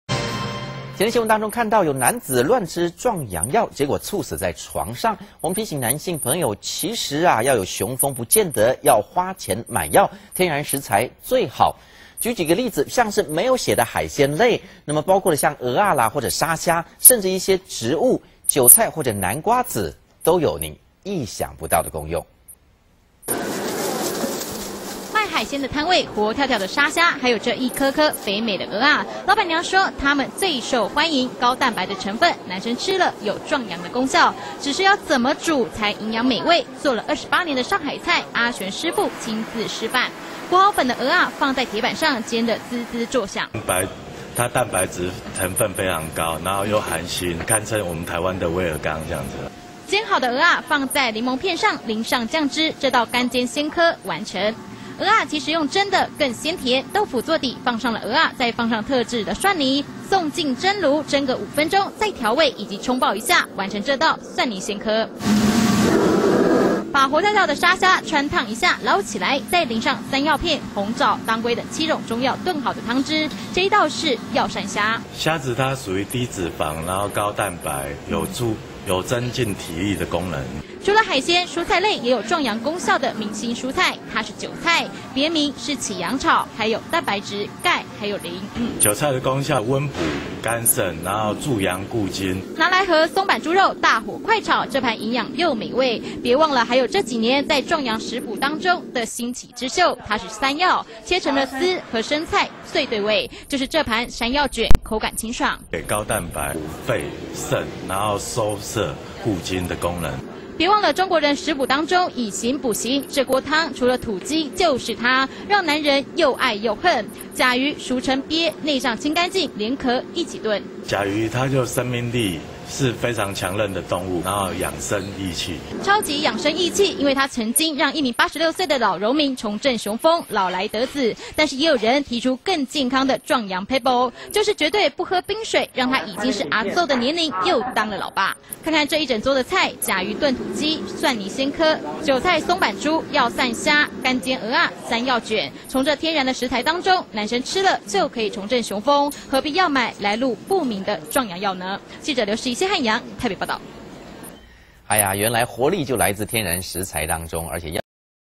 壯陽不用藥 食補就性福
新聞詳情

先前的新聞告訴您，有男子因為吃來路不明的壯陽藥，最後竟然在家中猝死。事實上，要有養身保健的效果，其實不需要吃藥，平常就可以從食物中，攝取該有的營養，也有重振的雄­風的功效，接下來，今天的頭條解碼我們就要告訴您，什麼東西，男生可以多吃，在保證安全的狀態下，也一樣可以追求幸福。

　　賣海鮮的攤位，活跳跳的沙蝦，還有這一顆顆肥美的蚵仔，老闆娘說，它們最受歡迎，高蛋白的成分，男生吃了有壯陽的功效，只是要怎麼煮？才營養美味，作了28年的上海菜，阿­玄師傅，親自示範，裹好粉的蚵仔，在鐵板上，煎得嗞嗞作響，煎好的蚵仔，放在檸檬片上，淋上醬汁，這道乾煎鮮蚵，完成，蚵仔其實用蒸的更鮮甜，豆腐作底，放上蚵仔，再放上­特製蒜泥，送進蒸爐，蒸個五分鐘，再調味以及蔥爆一下，完成這道蒜泥鮮蚵！

　　把活跳跳的沙蝦，川燙一下，撈起來，再淋上山藥片、紅棗、當歸等七種中藥燉好的湯汁，這是藥膳蝦；除了海鮮，蔬菜類也有壯陽功效的明星蔬菜，它是韭菜，別名是起陽草，含有­蛋白質，鈣還有磷，拿來和松板豬肉大火快炒，這盤營養又美味，別忘了，還有這幾年在壯陽食補中的新起之秀，它是山藥，切成絲，和生菜最對味，就是這盤山藥捲，口感清爽。